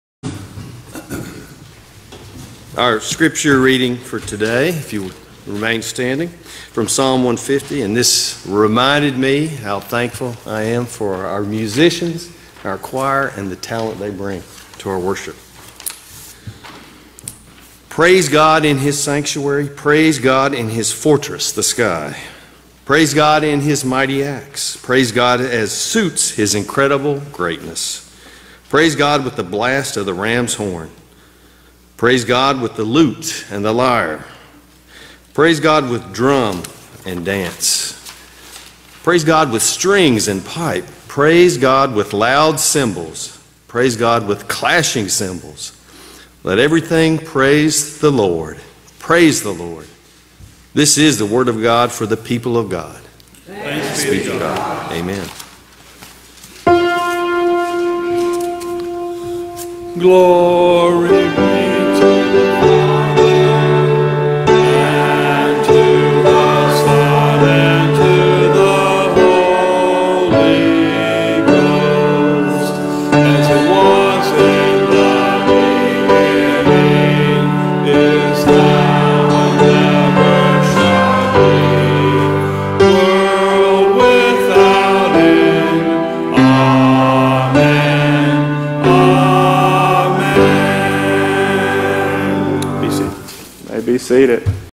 07172022 Scripture Reading: Psalm 150
The reading of Psalm 150 during the worship service at Trinity United Methodist Church in Big Stone Gap, Virginia.